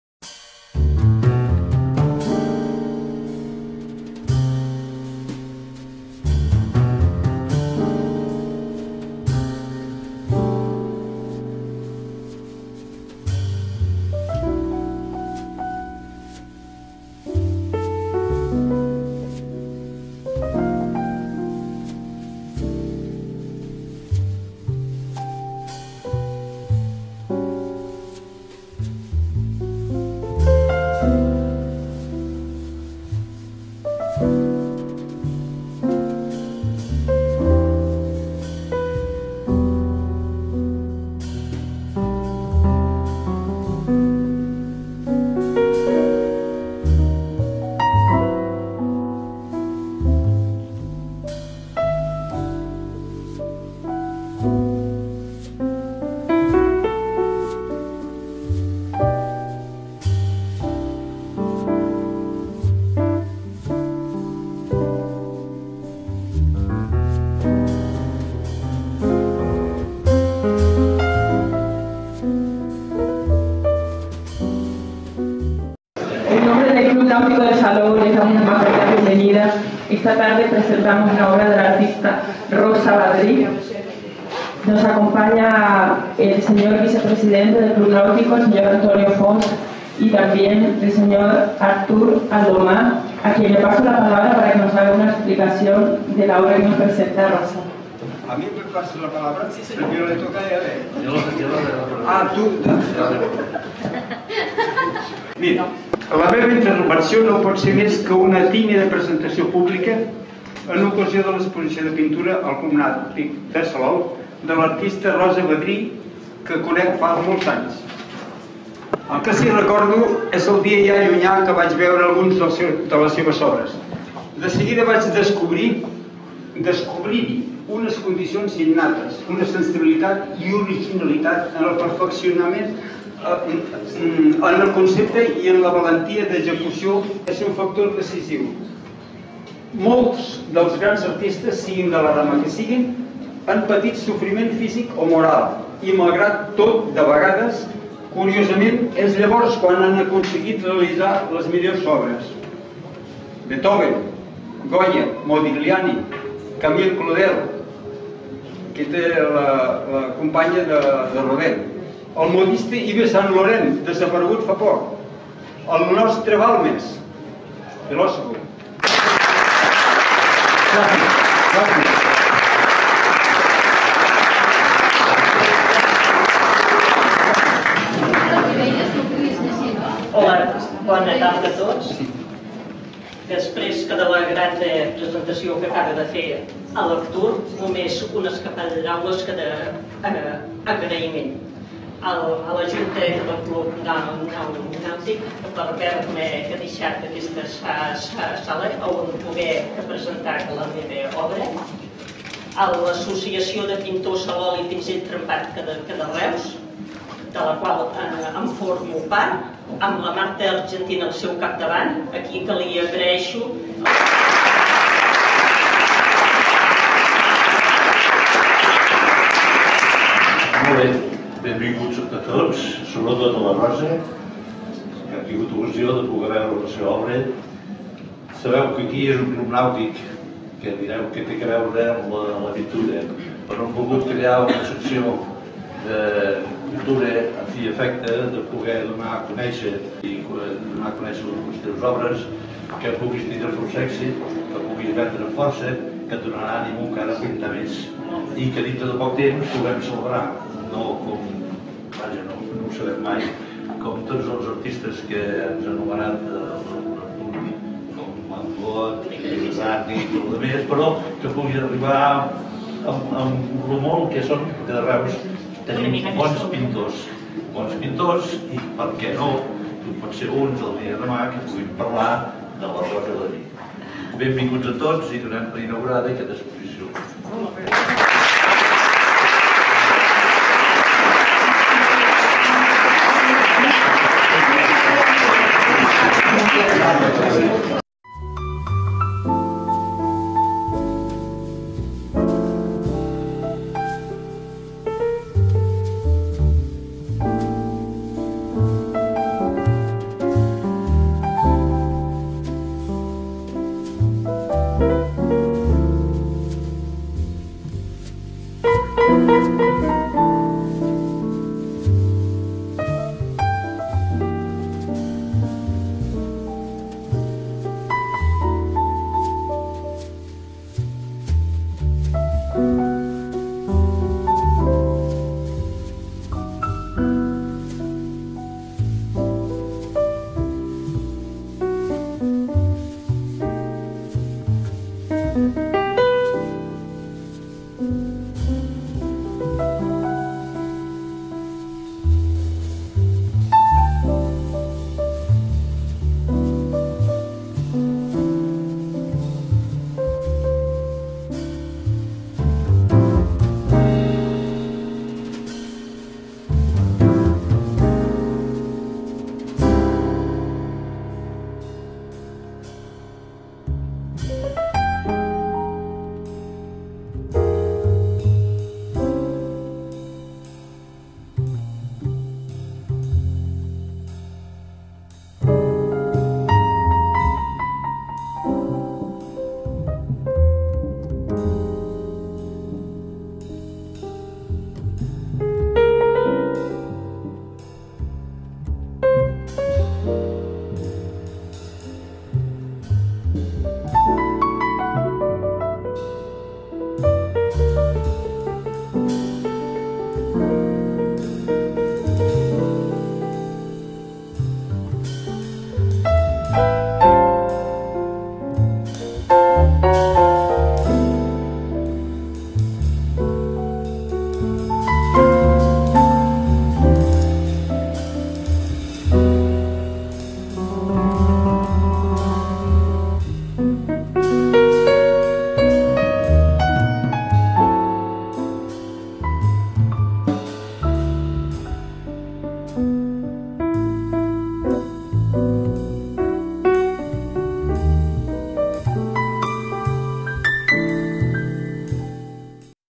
Expo de pintura de Rosa Vadrí en C.N.Salou
Inauguración de la exposición de pinturas a espátula de la pintora de Reus, Rosa Vadrí, en la Sala de Exposiciones del Club Nàutic Salou. En la presentación hizo una charla el eminente escultor Artur Aldomà elogiando la pintura de Rosa.
Los óleos de Rosa Vadrí destacan por su valentía y decisión en el uso de la espátula, con la que logra unos efectos impresionistas cargados de luz y color a la vez que de materia pictórica, en la que el relieve forma parte de la obra. A pesar de este aspecto matérico sus cuadros respiran transparencia y sutilidad. Sus temas, que pasan por la arquitectura de rincones de pueblos y tejados, a las marinas y escenas de puerto con embarcaciones, o las plantas cactaceas, poseen todas esa irradiación de la materia convertida en color.